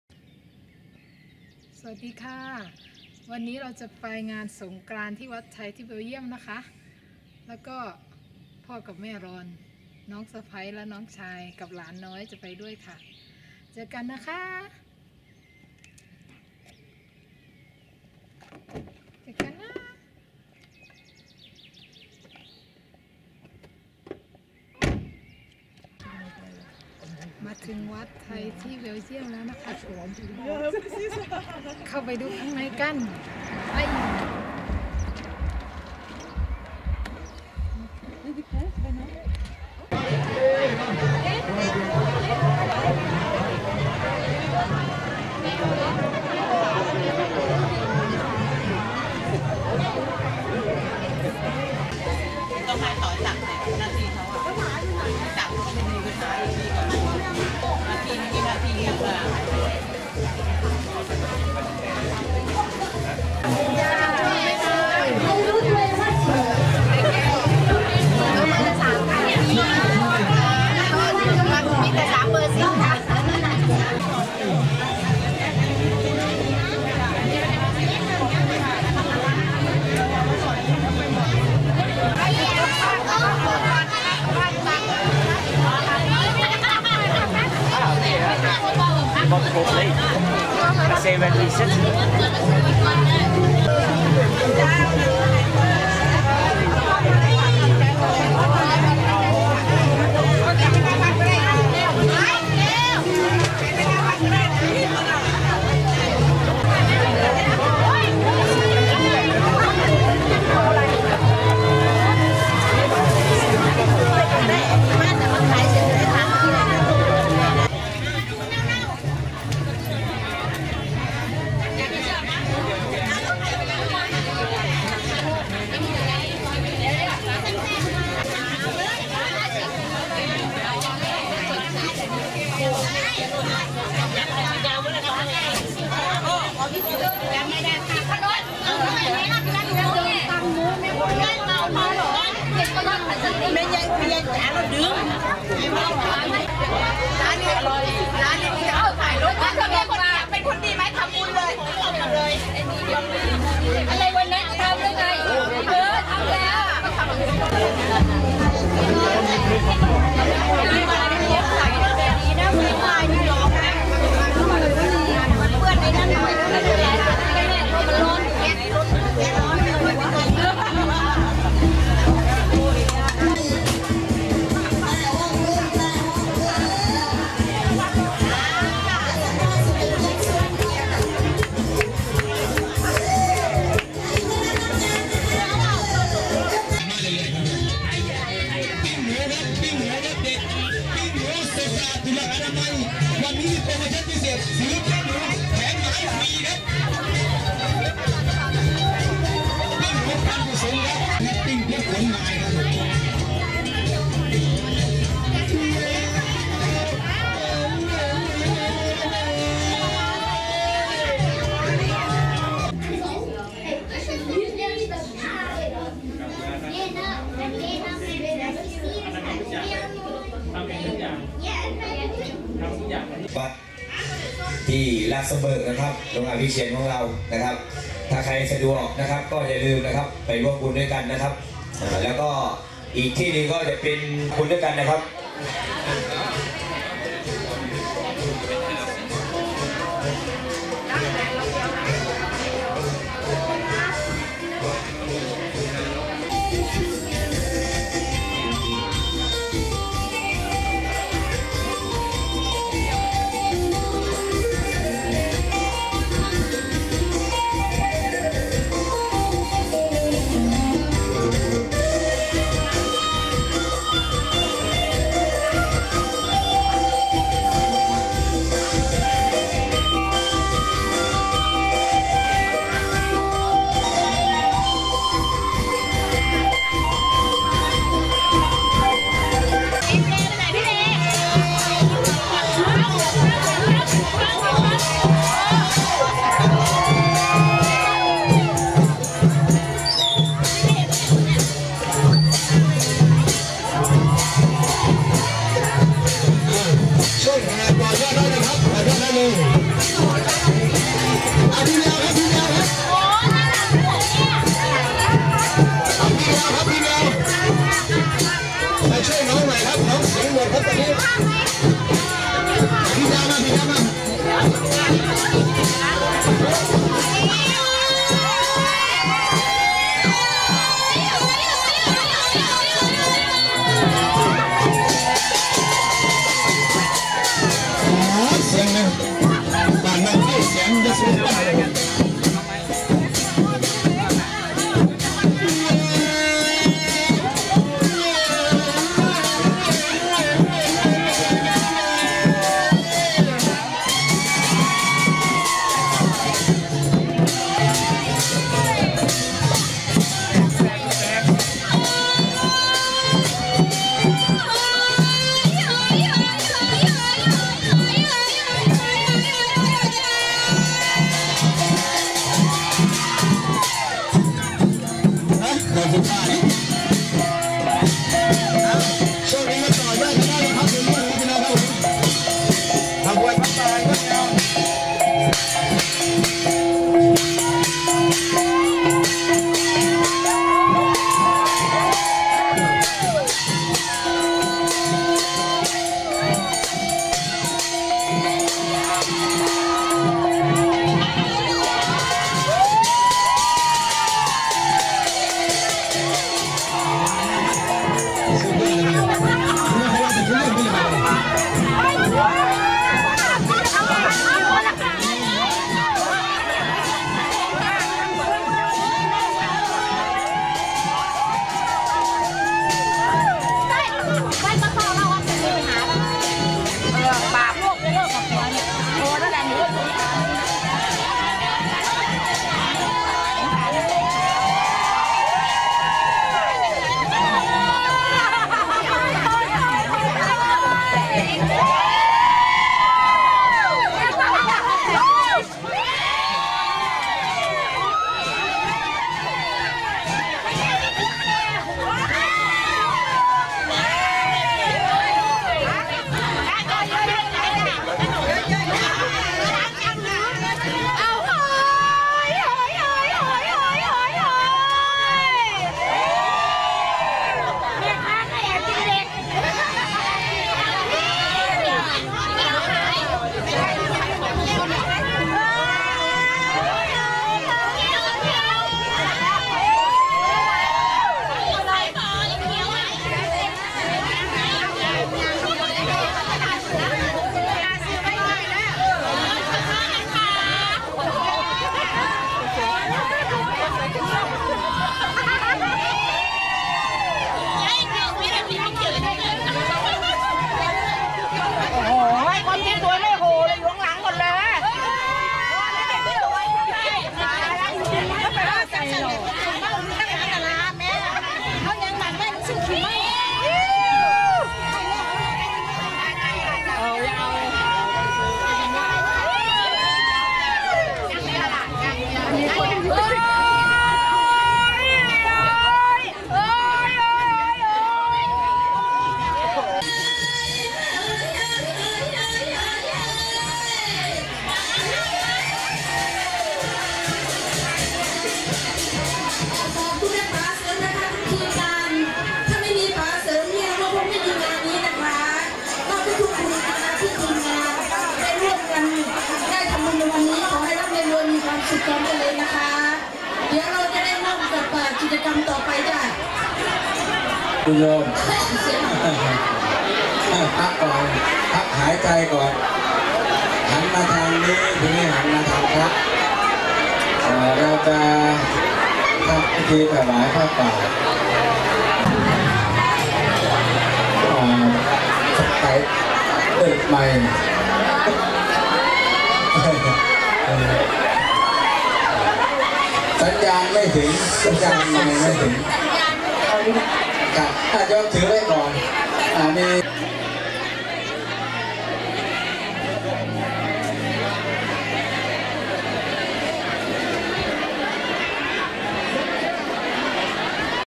Songkran Wat Buddharama, Sint Niklaas, Belgium 22 April 2018
วันนี้ไปงานวันสงกรานต์ที่วัดไทยในเบลเยี่ยมมาค่ะ ในงานก็จะมีประเพณีสงกรานต์แบบฉบับย่อนะคะเพราะเค้าจัดกันวันเดียว ก็ทำให้หายคิดถึงบ้านไปได้นะคะ บรรยากาศในงานก็เต็มไปด้วยความอบอุ่นนะคะสำหรับคนไทยที่อยู่ต่างประเทศอย่างพวกเรา ก็เป็นการรวมตัวกันของคนไทยที่เบลเยี่ยมและเนเธอร์แลนด์ด้วยค่ะ เพราะก็อยู่ไม่ไกลกันมากเท่าไหร่ค่ะ และคนเบลเยี่ยมในพื่นที่นี้เค้าก็พูดภาษาดัตช์กันค่ะ ก็มีสรงน้ำพระ ตักบาตร และมีผ้าป่าด้วยนะคะ สนุกมากเลยค่ะ และก็มีคนไทยเอาสินค้ามาขายหลากหลายมากค่ะ มีผักไทยที่เค้าปลูกกันที่นี่ และก็เบี้ยผัก เช่นพริก มะเขือ มะละกอ มีขนมหวานต่างๆ เหมือนเมืองไทยทุกอย่างเลยค่ะ  ปีหน้าถ้ามีโอกาสจะไปอีกค่ะ